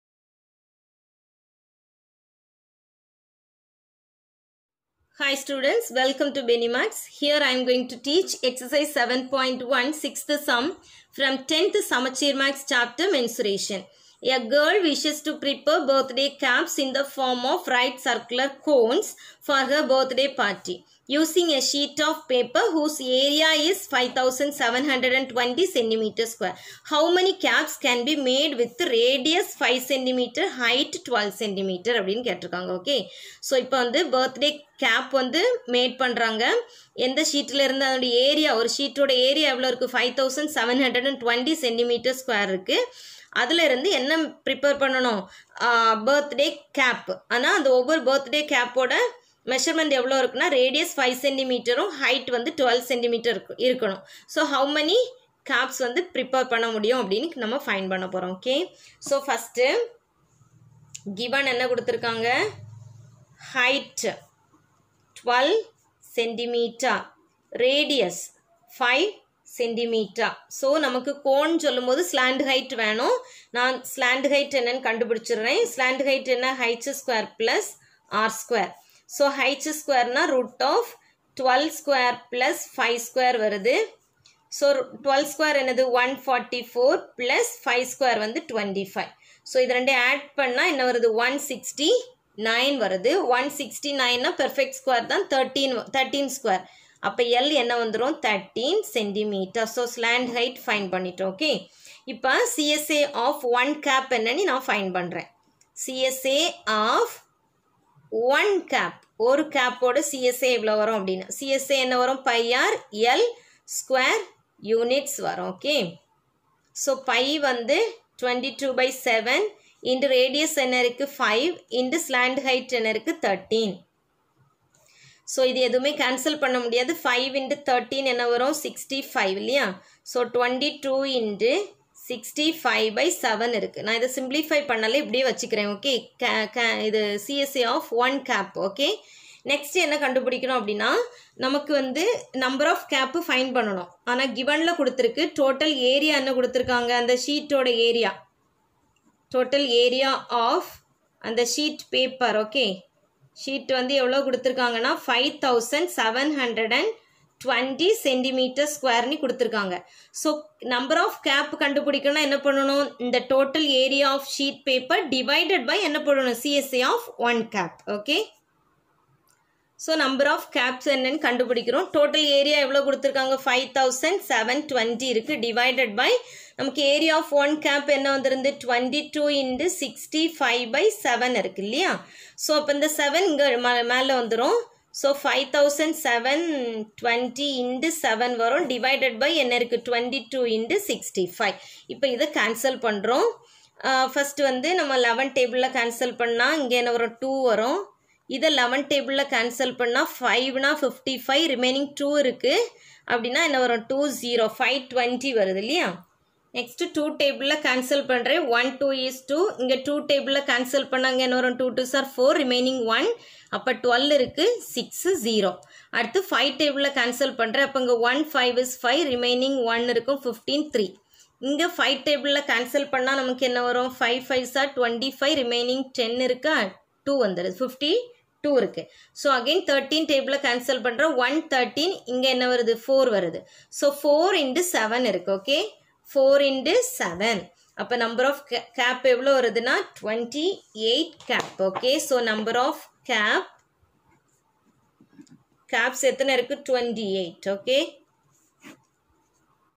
TN Class 10th Samacheer Maths | SSLC Maths | Exercise 7.1 Sum 6 | Mensuration | New Syllabus
Welcome to Beny Maths. In this video I have explained 10th Samacheer Maths Exercise 7.1 sum 6 from Chapter 7  Mensuration .
10thmathsexercise7.1 sum 6
   Presented by,
        Y. Beny Andrew, MSc, MEd. (Maths)